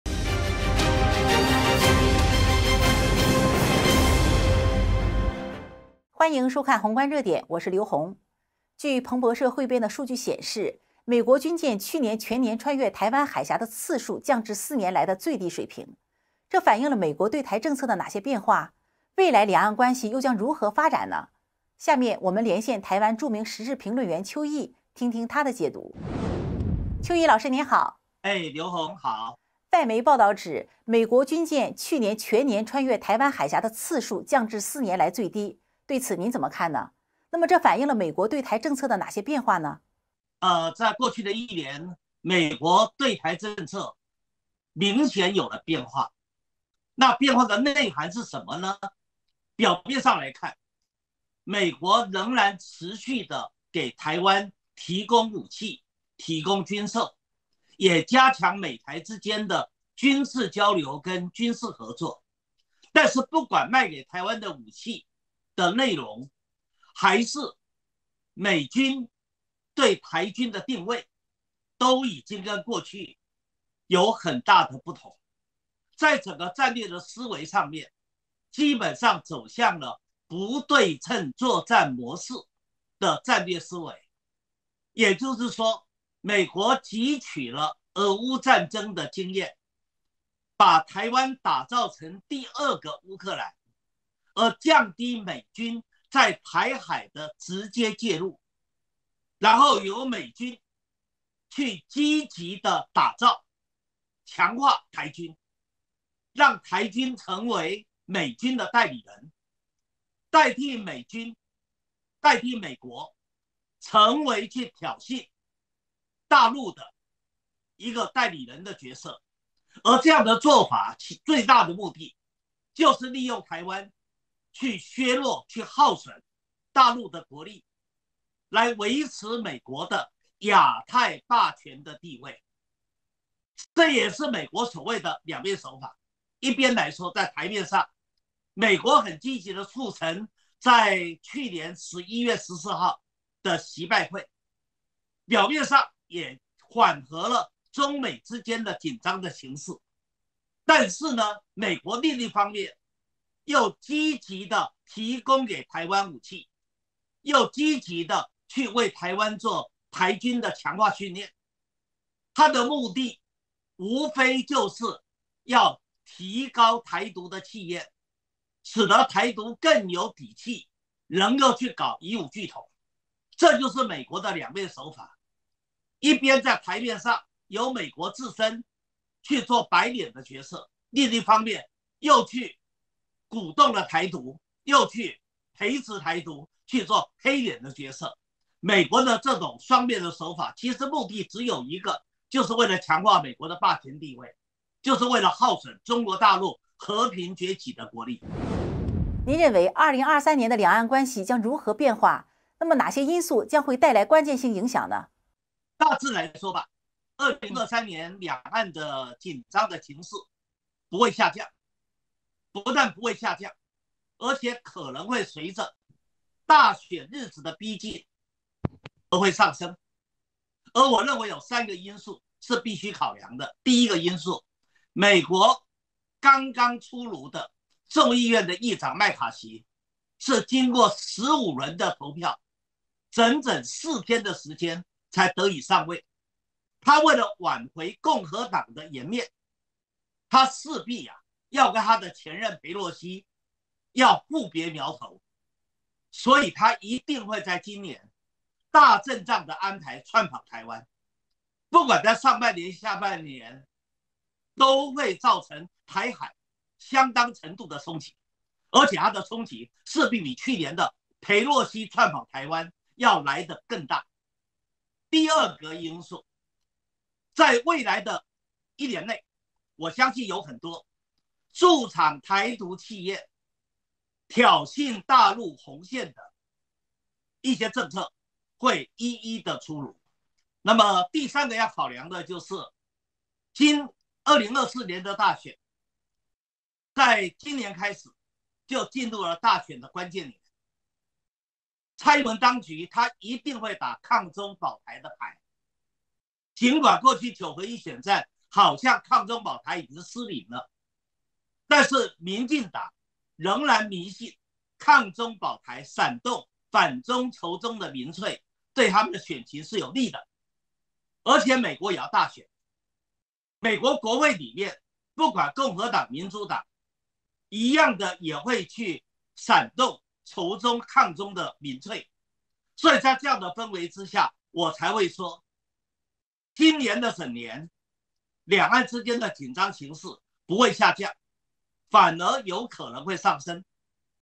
邱毅談美對台政策變化和2023兩岸關係走向
紅觀熱點 邱毅談美對台政策變化和2023兩岸關係走向